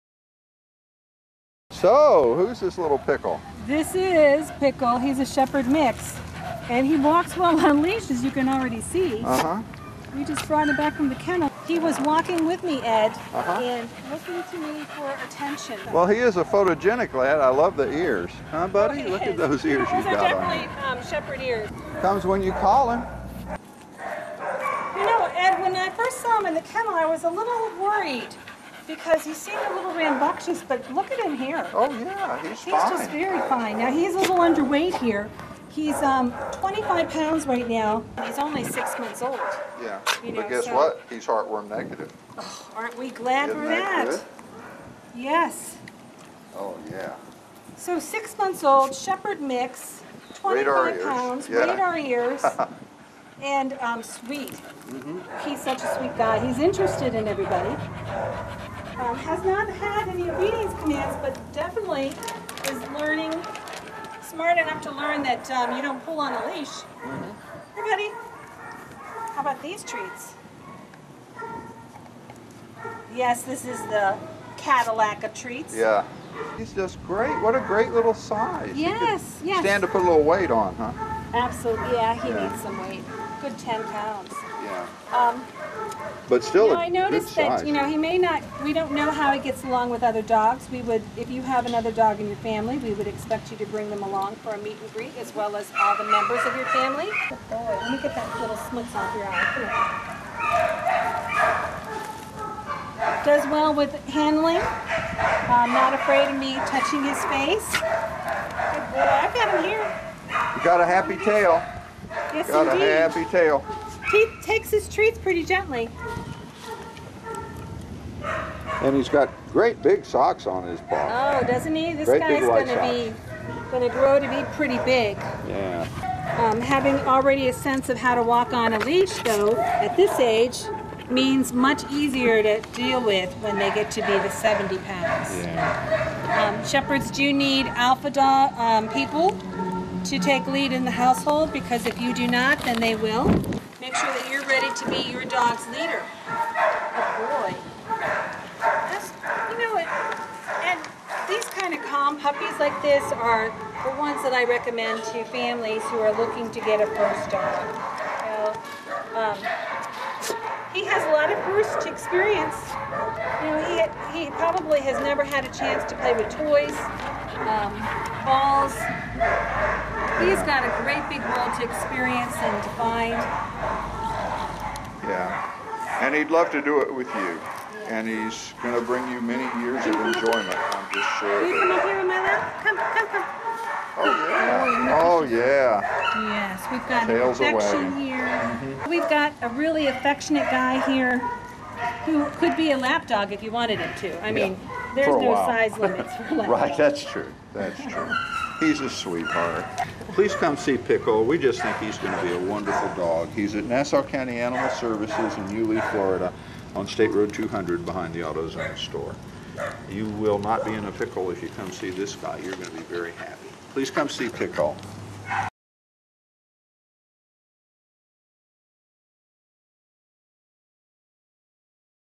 Please rescue happy Pickle!
Pickle is a handsome Shepherd mix who walks very well on his leash for a puppy, has a big smile and plenty of affection!